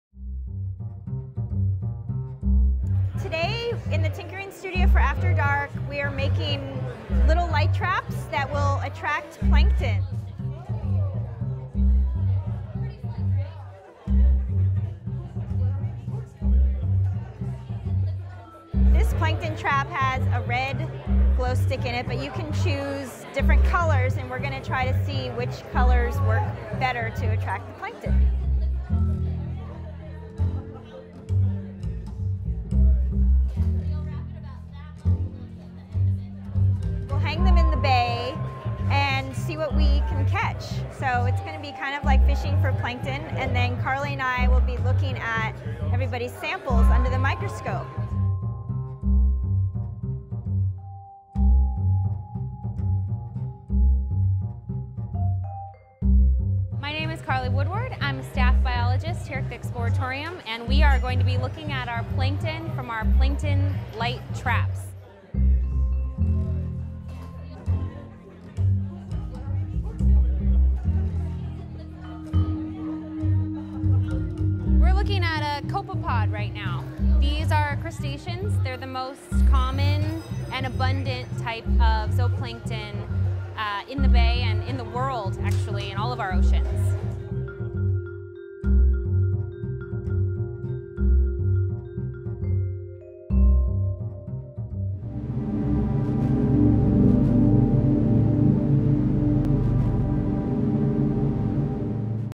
Plankton Light Traps I Activity I Exploratorium
The Bay is filled with microscopic creatures called plankton. Many of these creatures rise to the surface at night to feed when their predators can't see them. On December 5th, 2013 After Dark guests got a chance to make their own light traps to collect plankton using a water bottle and a glow stick. Take a look at their catch with Exploratorium Biologists, Denise King, Karli Woodward and Jen Frazier.